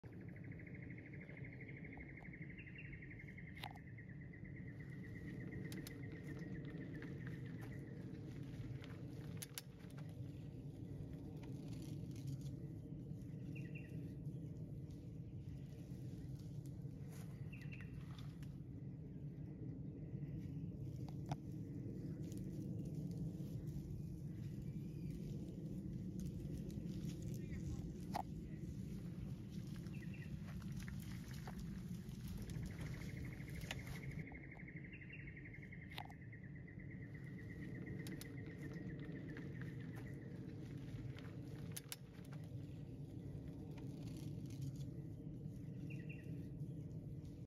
Incredible encounter on Australian trail | 4K [2023]
1 meter long Goanna spotted on mountain bike track in the Redlands Track Park Scribbly Gums Conservation Area.